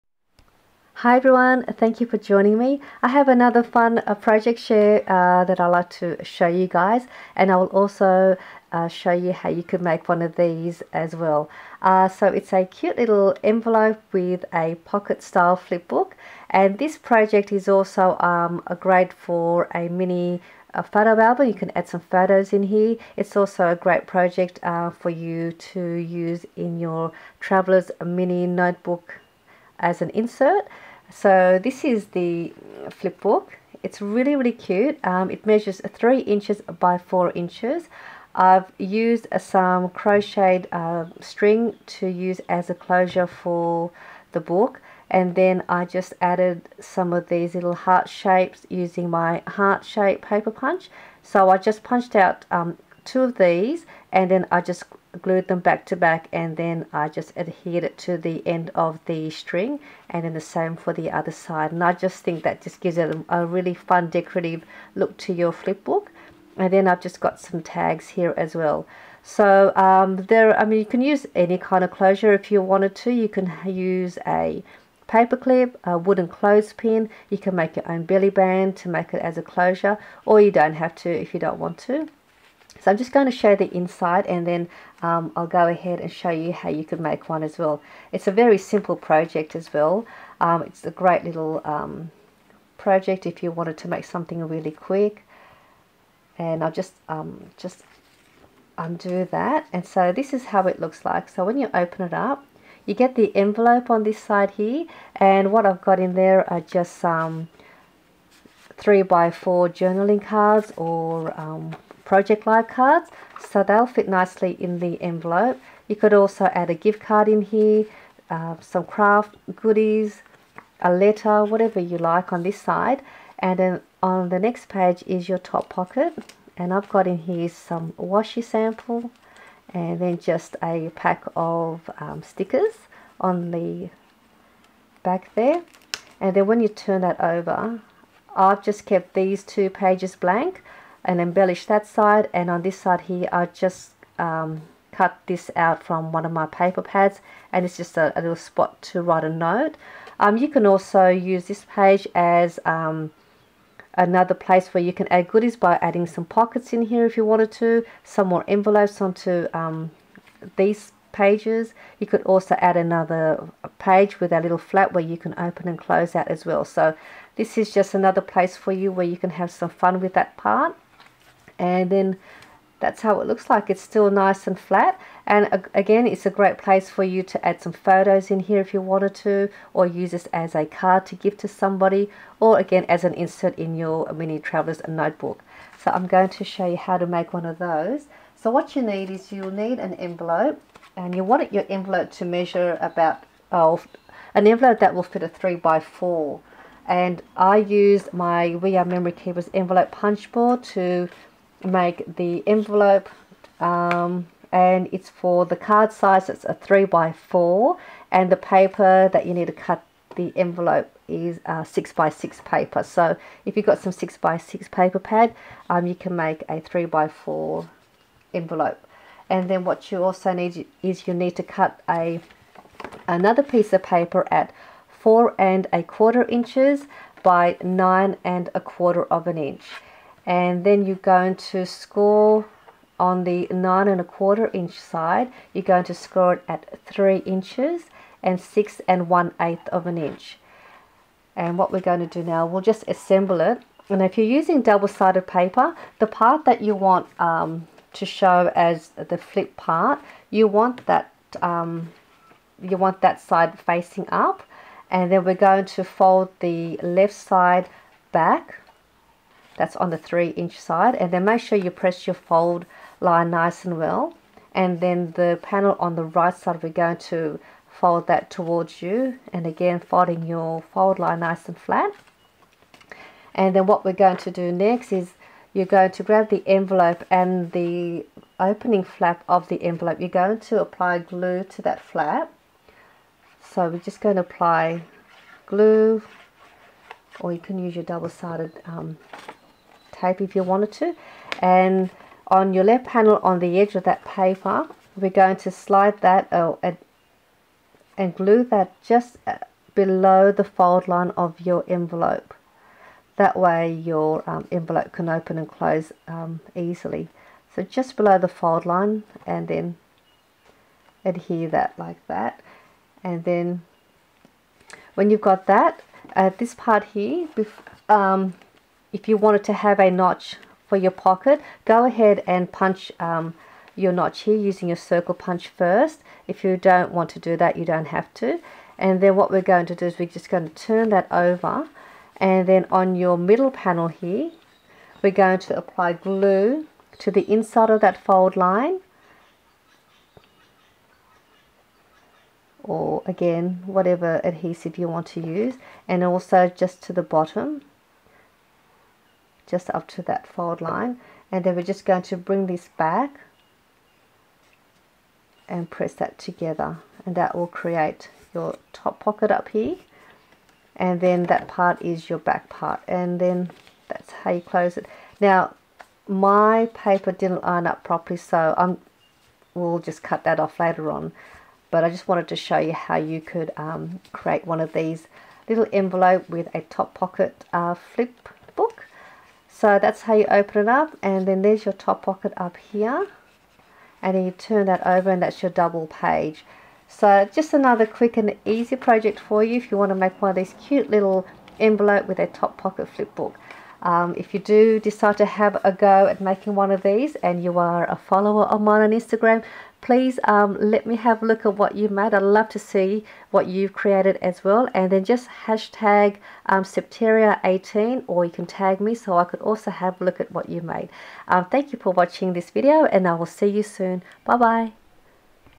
Envelope & Pocket Flipbook/TUTORIAL
Create this simple Envelope & Pocket Flipbook that you can also use as a mini photo album or as an insert for a mini pocket Travelers Notebook.

Measurements:
Envelope Size : 3"x4"
Paper for pocket flip: Cut paper at 9 1/4" x 4 1/4"
On the 9 1/4" score at: 3" and 6 1/8"

MY LINKS
ETSY SHOP